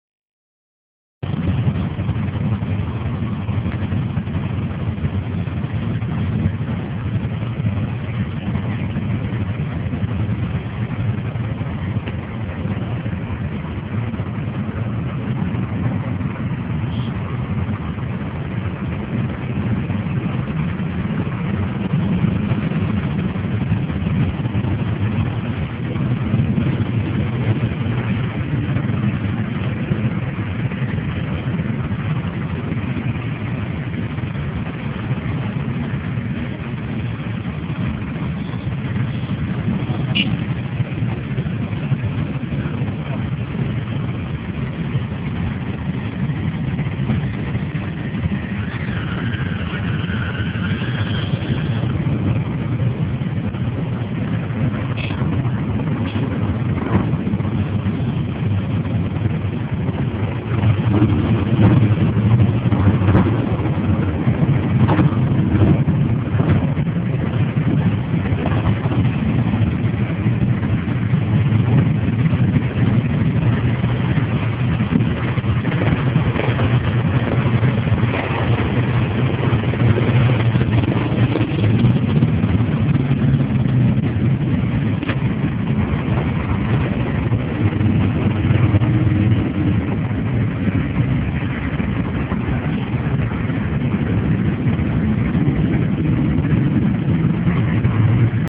LOS MOTEROS DE ASTURIAS DE LUTO
Por el fallecimiento del *** motero CHINORRIS *** sus compañeros de toda España le rindieron un merecido homenaje.
Llegada al tanatorio de Cabueñes-Gijón de la FAMILIA MOTERA.
Poco más tengo que añadir, solamente que soy dura y que lo ví en persona y terminé llorando.